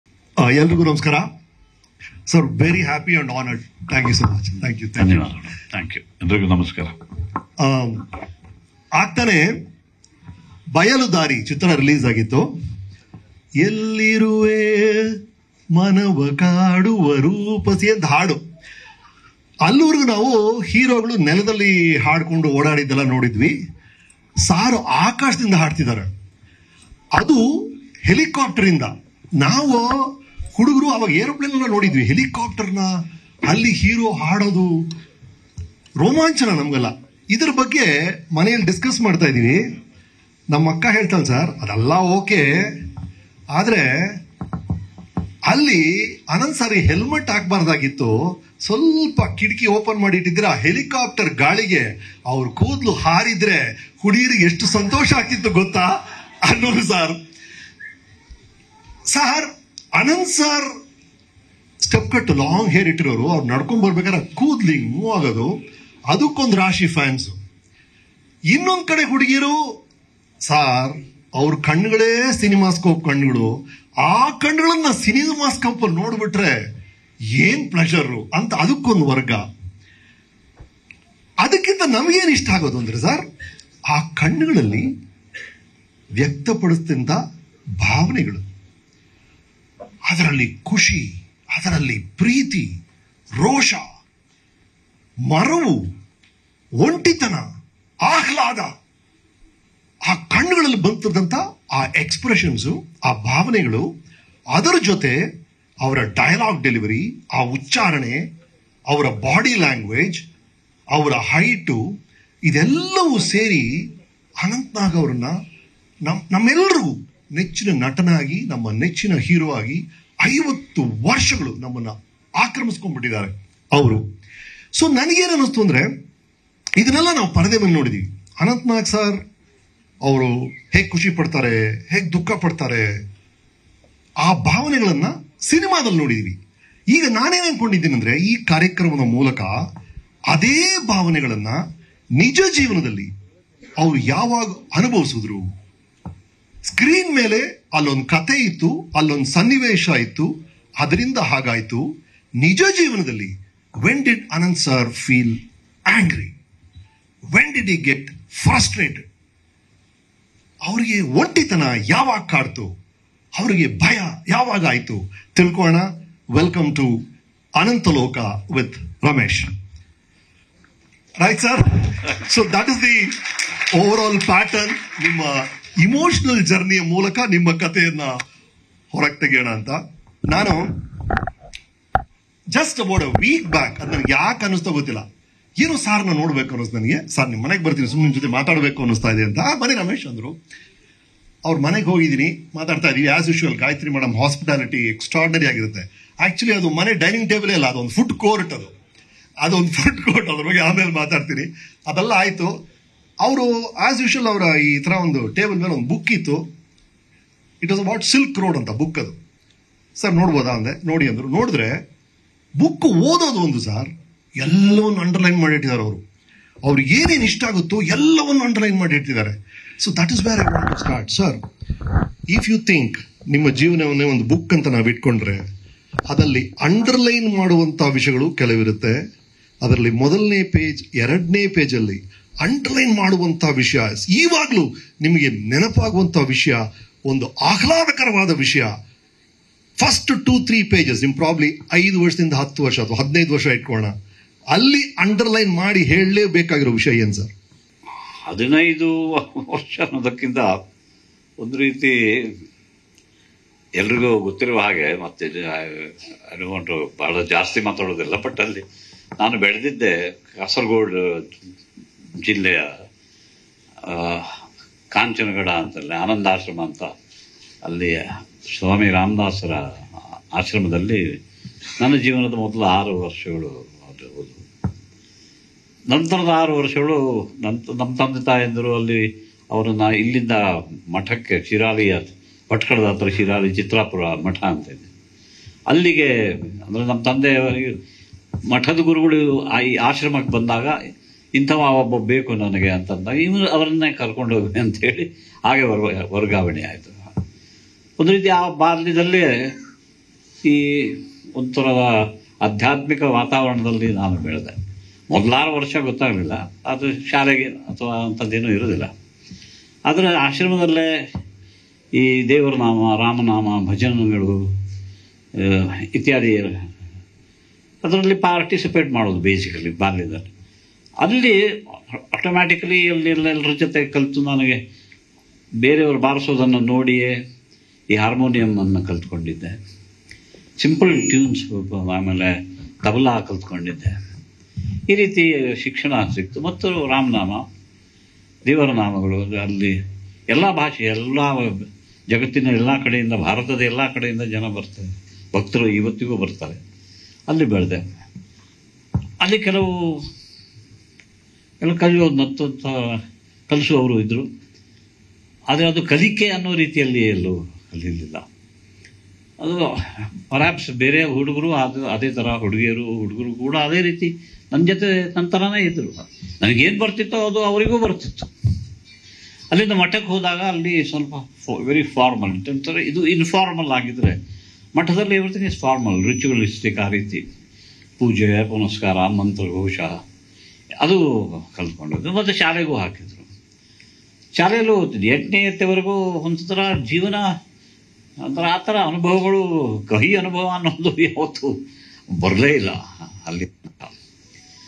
AnanthNag Interview | ಅನಂತ್​ನಾಗ್​​ಗೆ ಕೋಪ, ಸಿಟ್ಟು ಬರೋದು ಯಾವಾಗಾ? | Ramesh Aravind
News18 Kannada Live | ನ್ಯೂಸ್ 18 ಕನ್ನಡ ಲೈವ್
Karnataka | ಕರ್ನಾಟಕ
Kannada | ಕನ್ನಡ